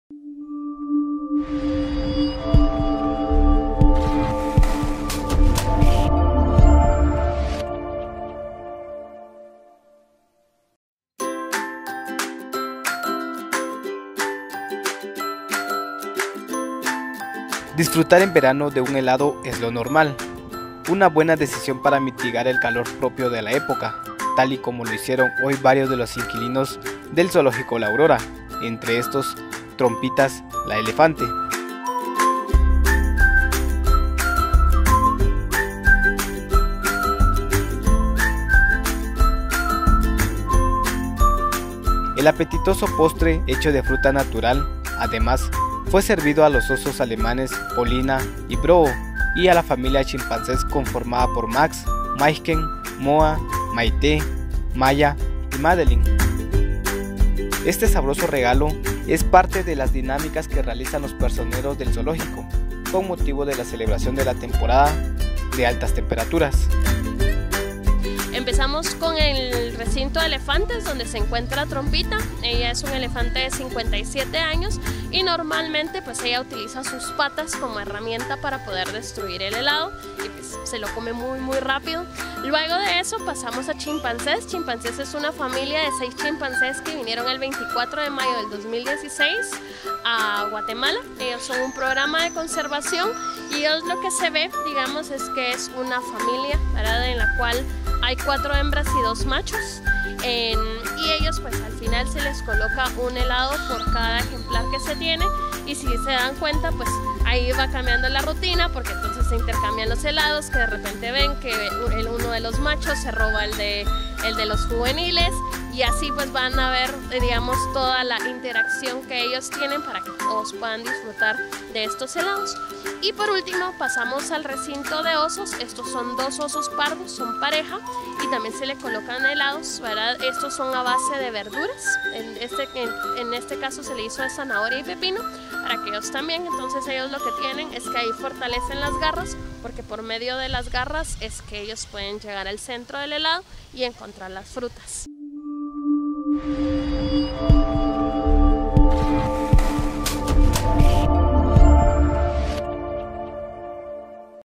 Zoológico sirve un helado de frutas a “Trompitas” y a 8 de sus vecinos
Disfrutar en verano de un helado es lo normal. Una buena decisión para mitigar el calor propio de la época, tal y como lo hicieron hoy varios de los inquilinos del Zoológico La Aurora, entre estos Trompitas, la elefante.

El apetitoso postre, hecho de fruta natural, además, fue servido a los osos alemanes, Polina y Broho; y a la familia de chimpancés conformada por Max, Maijken, Moa, Maite, Maya y Madelene.

Este sabroso regalo es parte de las dinámicas que realizan los personeros del zoológico con motivo de la celebración de la temporada de altas temperaturas.

La obra se realizó en tres recintos, en el de elegantes donde se encuentra trompitas, en el de la familia de 6 chimpancés y en el recinto de la pareja de osos.